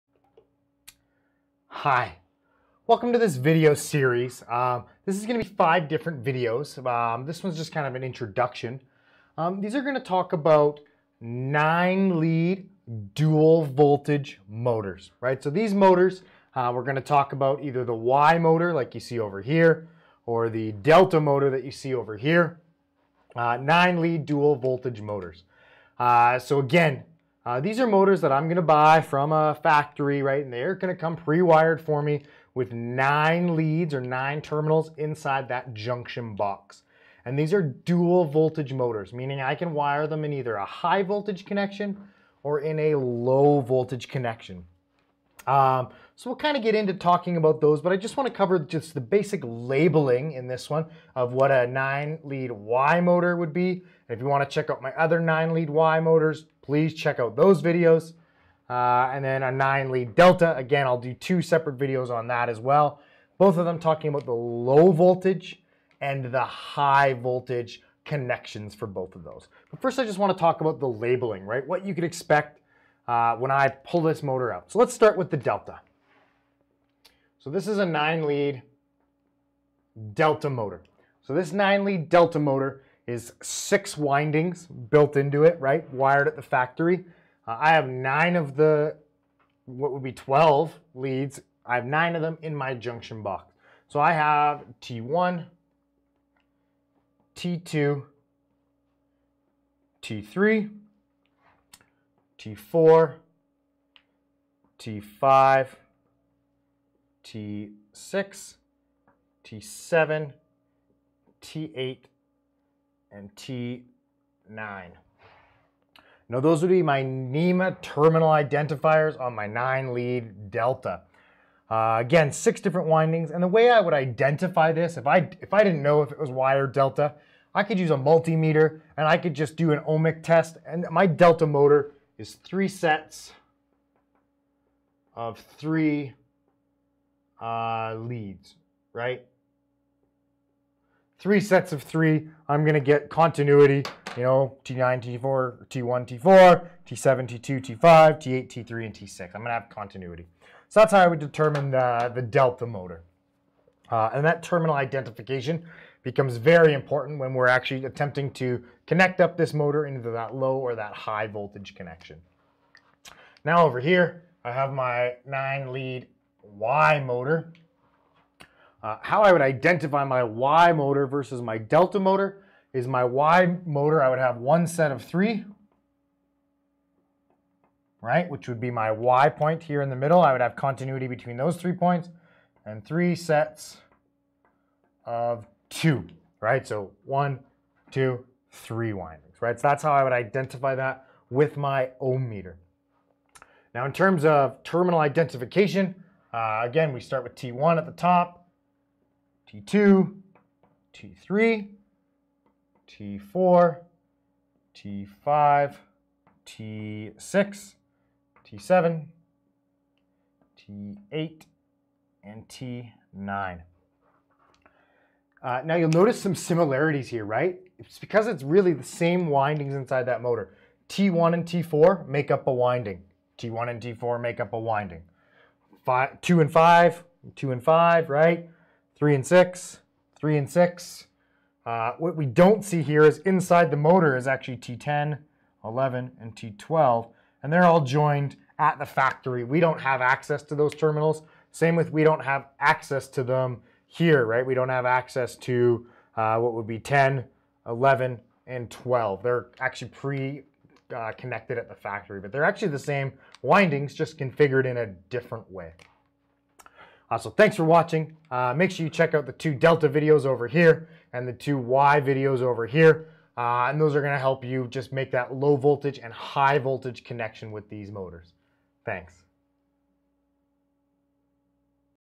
Dual Voltage Motors Introduction and Terminal Identification
This is video 1 in a series of 5 videos.

This video covers the terminal identification that need to be made for a Wye and Delta 3-phase 9-lead dual voltage motor.

This information can be used to perform a motor lead ID and make the connection required to get it running in the proper configuration.